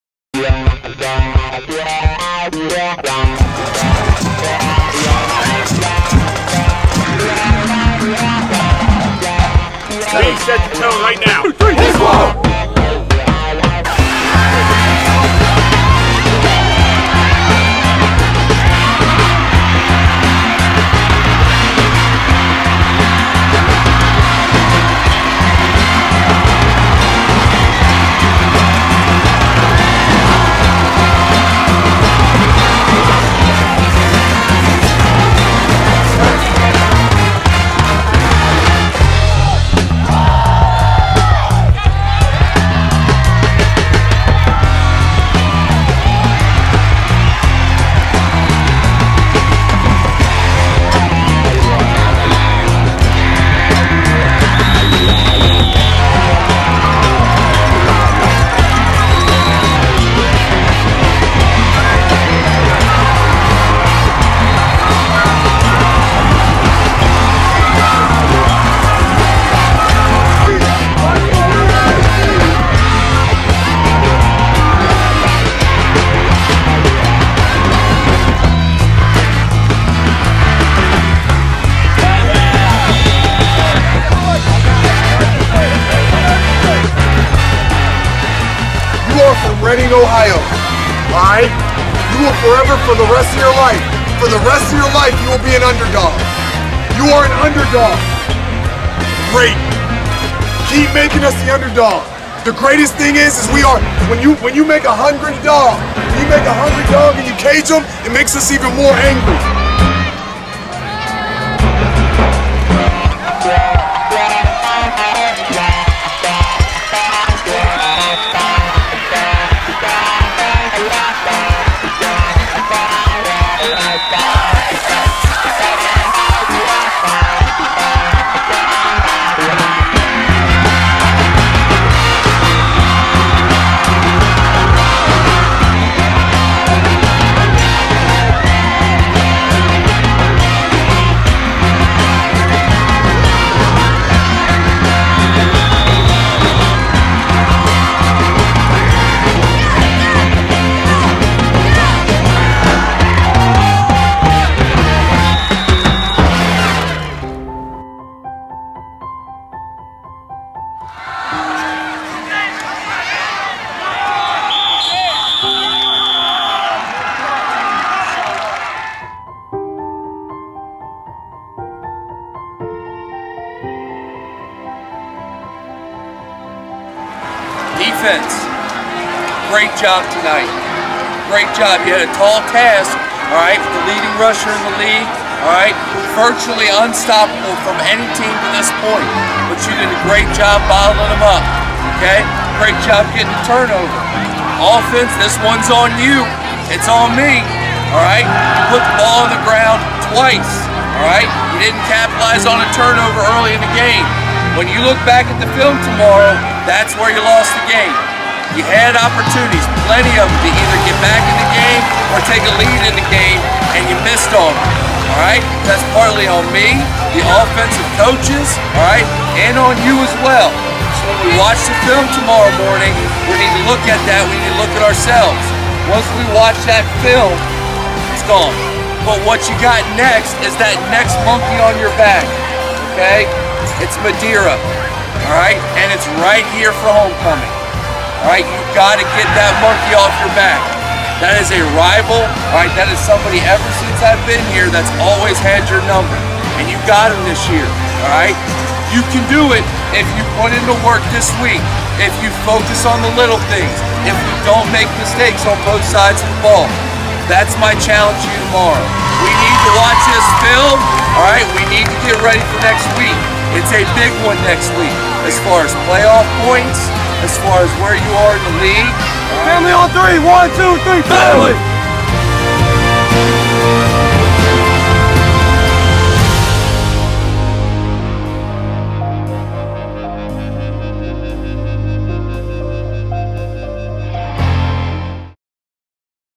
WYOMING AT READING 9/17/21 4K ULTRA HD
Images and impressions from Wyoming at Reading high school football game September 17 2021. (Filmed/uploaded in 4K 2160p/30fps video quality by Cincatt Productions).
Final Score: Wyoming 24 Reading 14